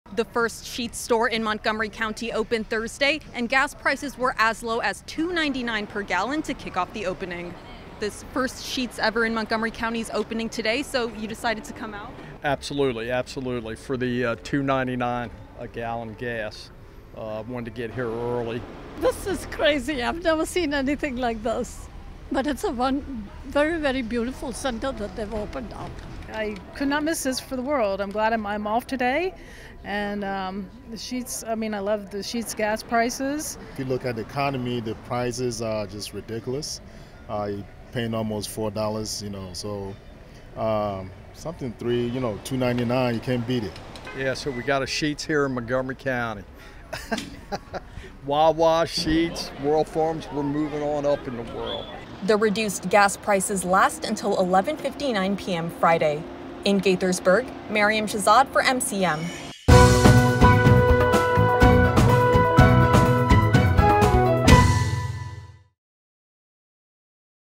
First Montgomery County Sheetz Opens in Gaithersburg
The county’s first Sheetz opened in Gaithersburg.

The restaurant and convenience chain has a new location at 751 Progress Way. A grand opening was held on Thursday, and gas prices were as low as $2.99 per gallon to kick off the opening.

The reduced gas prices last until 11:59 p.m. Friday. Plus, 10 cents from every gallon sold over the period will be donated to the Special Olympics of Maryland.